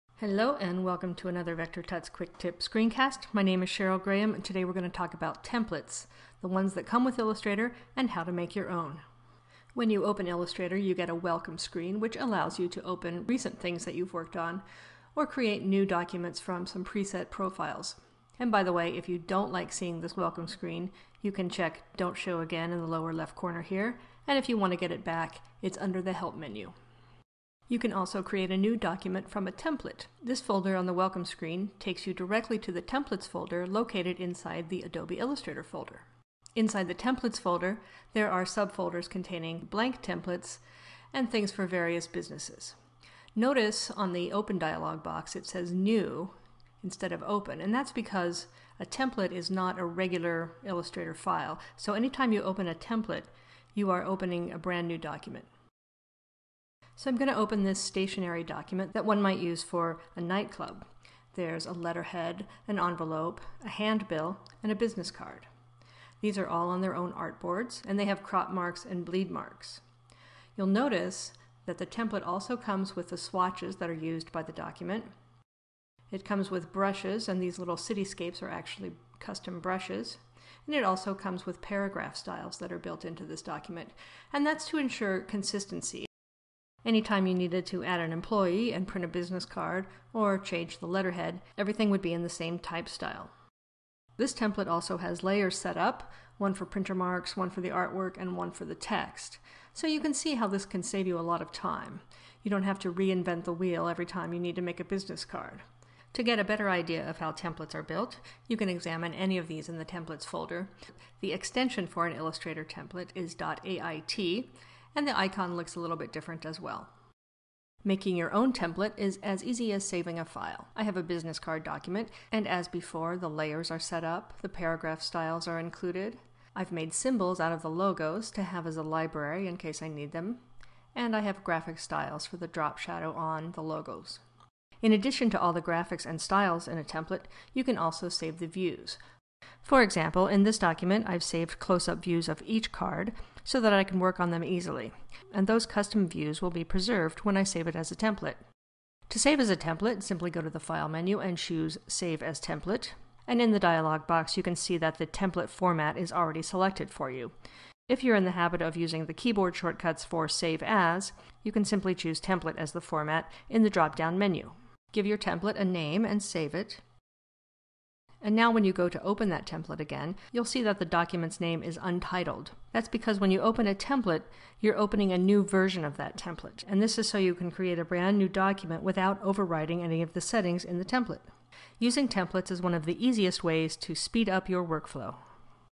How to Make and Use a Template in Illustrator
Illustrator comes with tons of templates to help speed up your workflow. But did you know that it's also super easy to make your own? In today's Video Quick Tip you will learn how to make Illustrator templates for various designs and layouts.